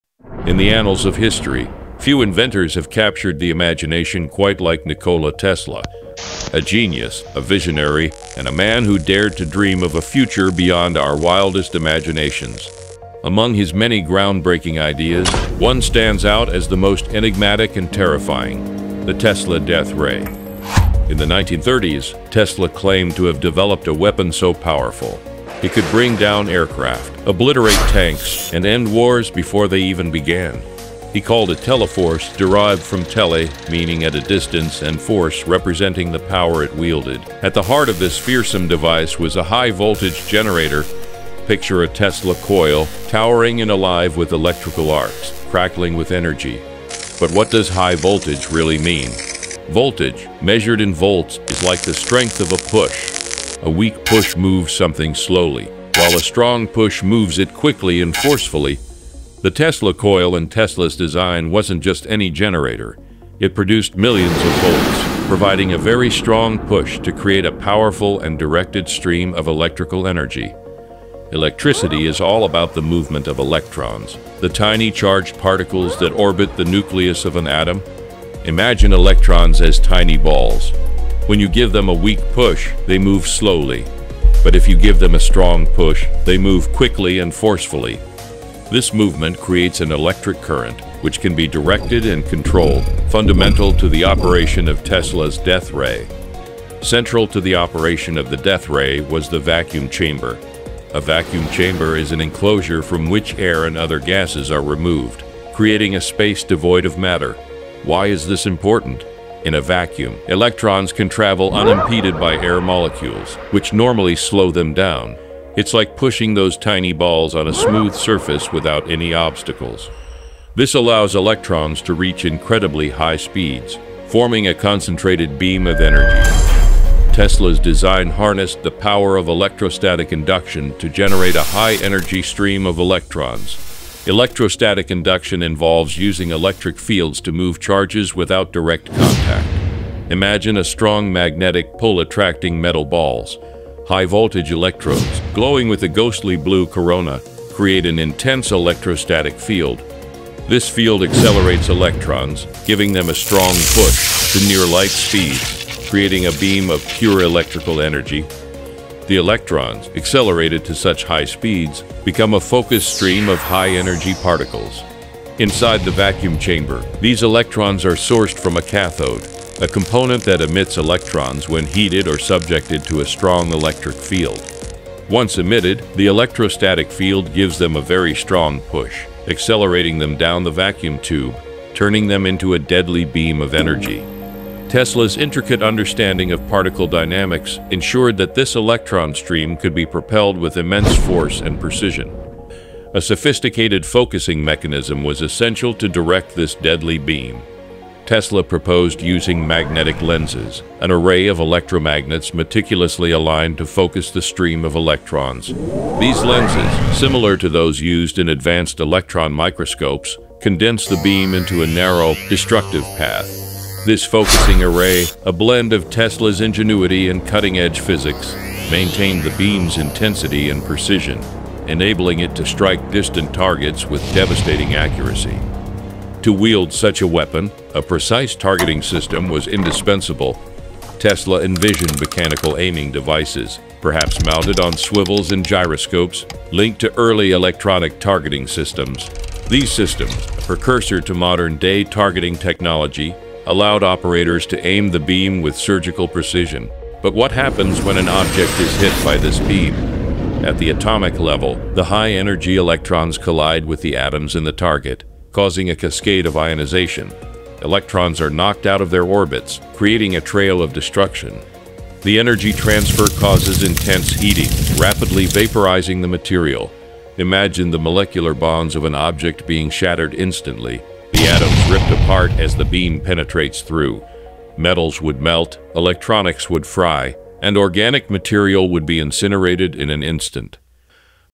How the Tesla Death Ray Might Have Been Built
Nikola Tesla, a visionary inventor, dreamed of a weapon to end wars - the Tesla Death Ray. This video explores the science behind this legendary device:

How high-voltage electricity creates a powerful, directed beam.
The role of vacuums and electron flow in maximizing energy.
Focusing the beam with magnetic lenses for pinpoint accuracy.
The challenges of powering and controlling such a weapon.
Tesla's vision for a war-free future through ultimate deterrence.
The lasting impact of Tesla's ideas on directed energy technology.
Was the Death Ray real? Join us as we delve into the mystery and legacy of this invention!